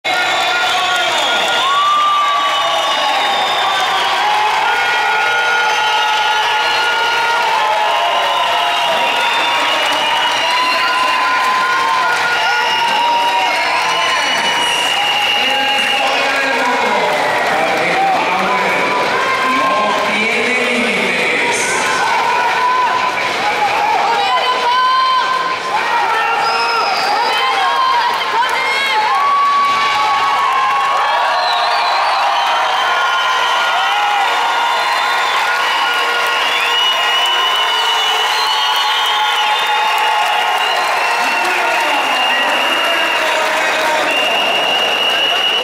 Carl Yngvar Christensen 465 kg / 1025 lbs World Record Squat - World Games 2013
20kg+ world record junior @ +120